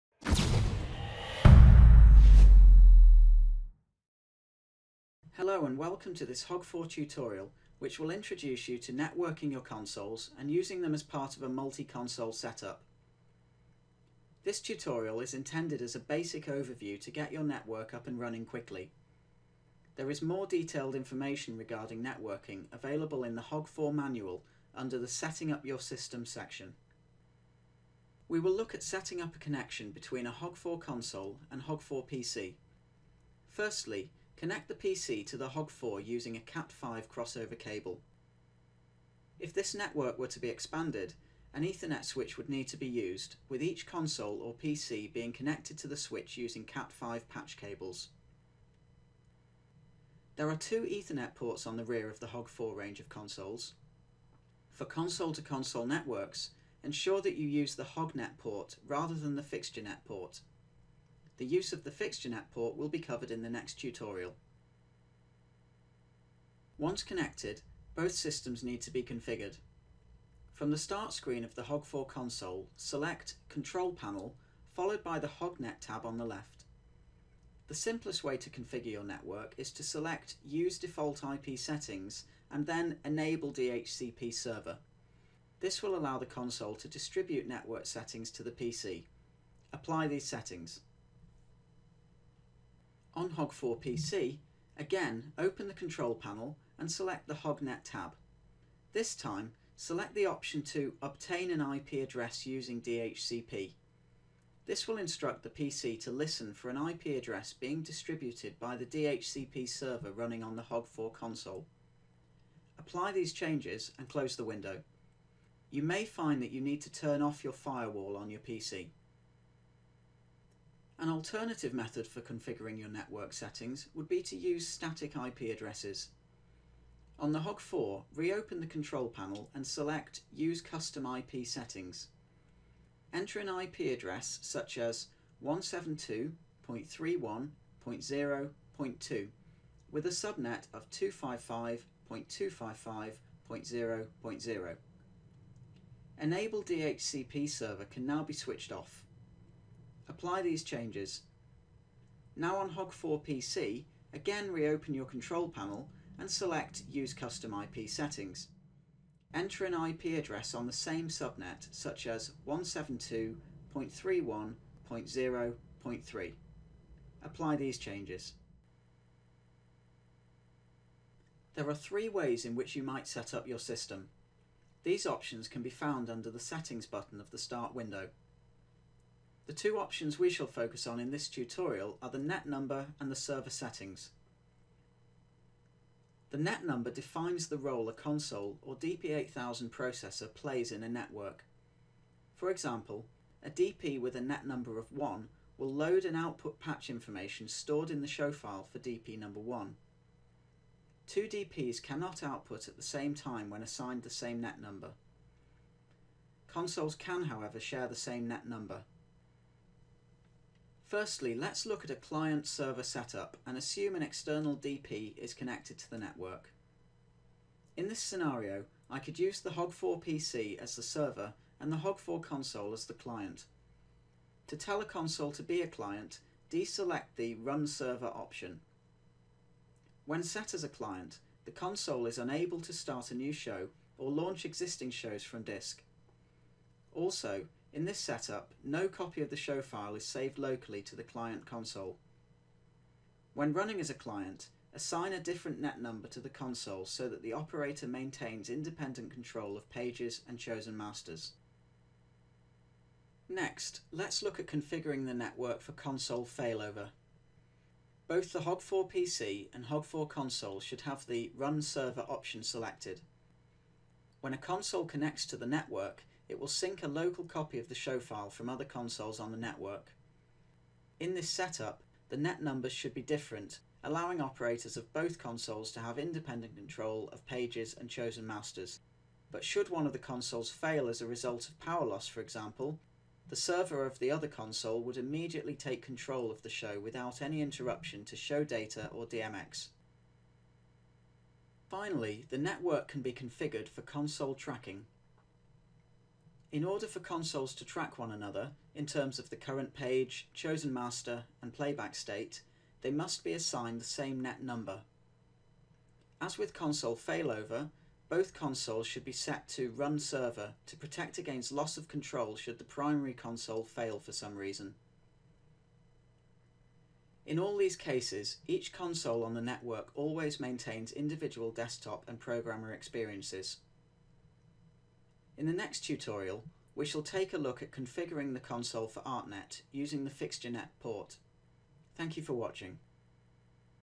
Hog 4 Tutorial 11: Multi Console Setup
Networking consoles and using them as part of a multi console setup.

Visualization software for this series is provided by LightConverse